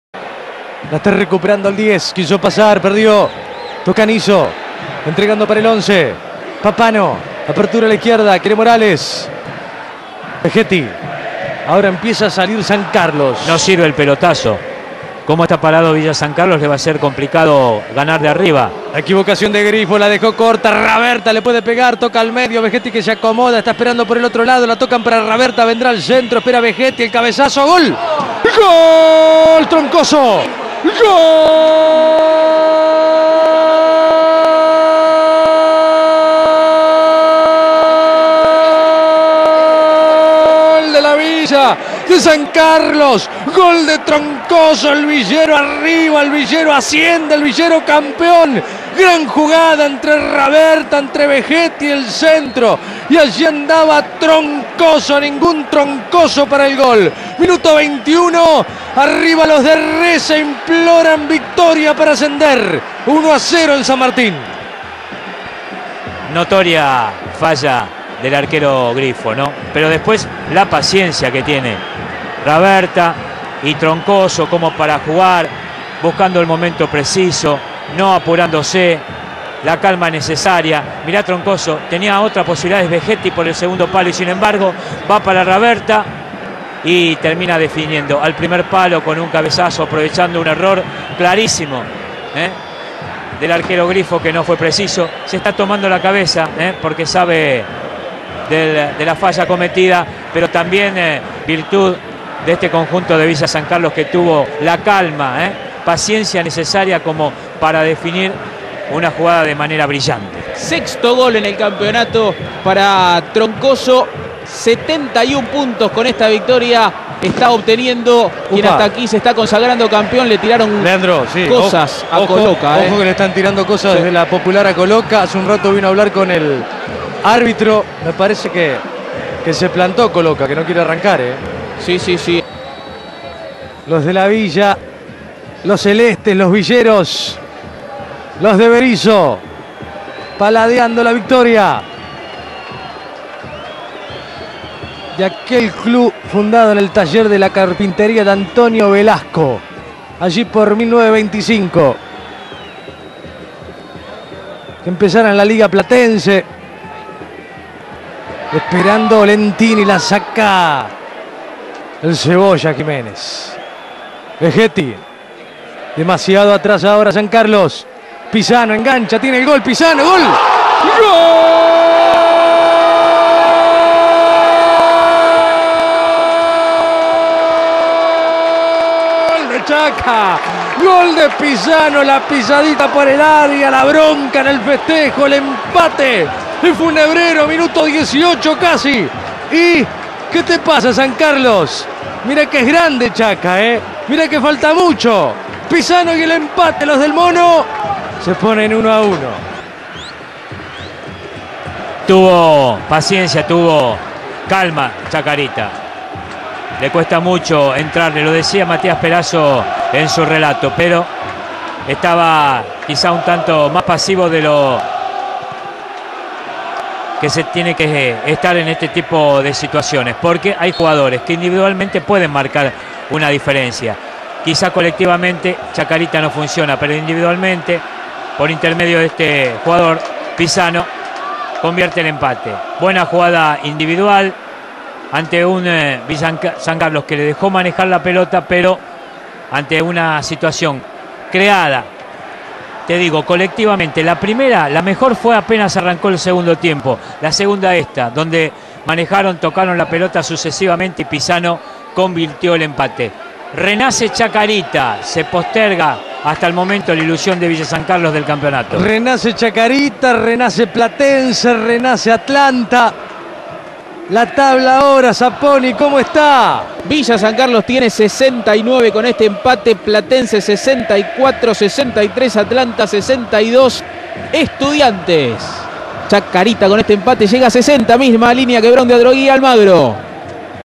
Chacarita 1 Villa San Carlos 1 (Relato Martin Perazzo) Primera B Metro 2012/13 Los goles (18/5/2013)
Los goles del partido con relato de Martin Perazzo Tyc Sports